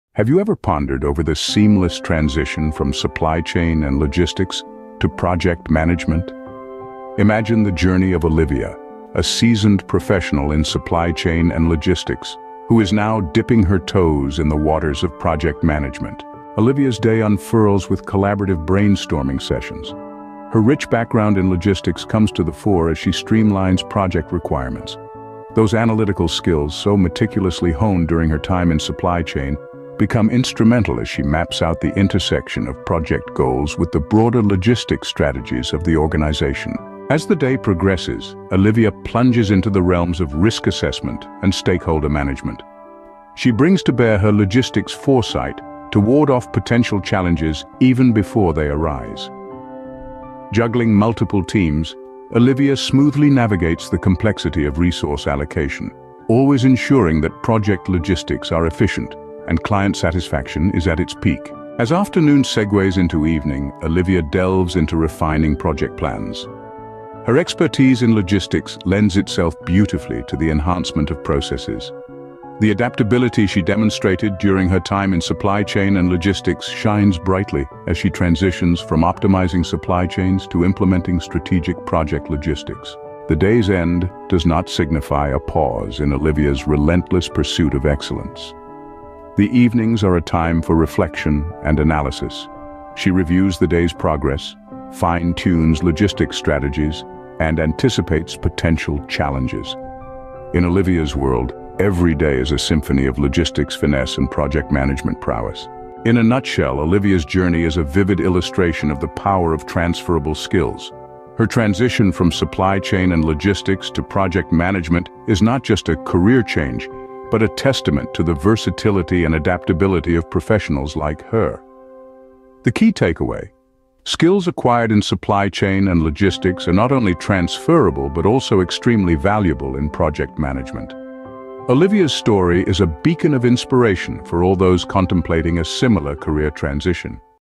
Olivia's Journey From Logistics Pro to Project Management
Olivia, a skilled Supply Chain/Logistics Professional, seamlessly transitions into project management. Mornings involve strategic planning, afternoons optimize processes, and evenings entail reflection and logistics strategy refinement in her transformative journey.